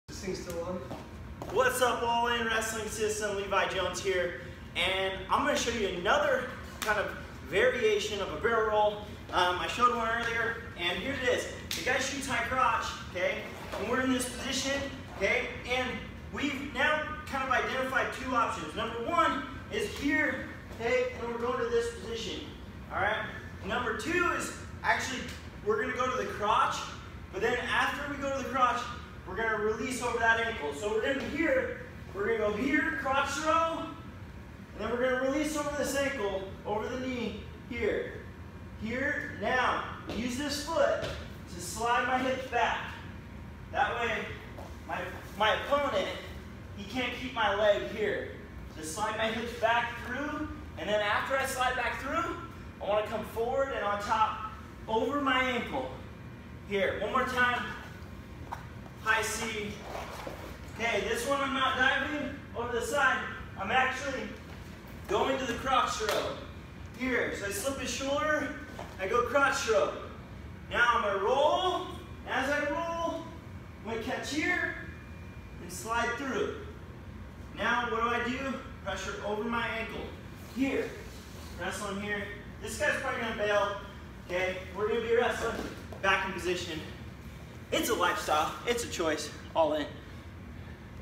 Sneaky way to reverse opponent from his shot..! Crotch Throw Defensive Scramble
This is a great scramble technique when you have eliminated all solid options first and your ready to take the match to a scramble. Imagine a back door that is opening to an opportunity to score lots of points.